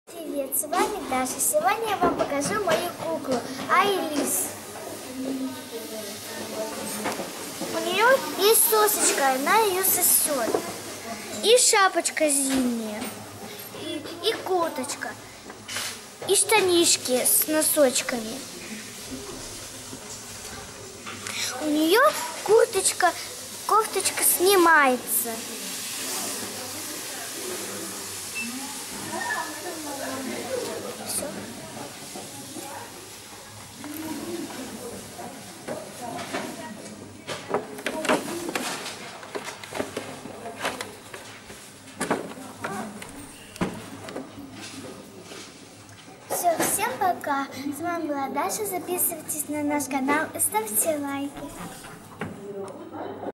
КУКЛА Элис от ANTONIO JUAN! 64с
Сегодня у меня кукла Элис от от  ANTONIO JUAN которая очень похожа на ребеночка и одета в зимнюю одежду.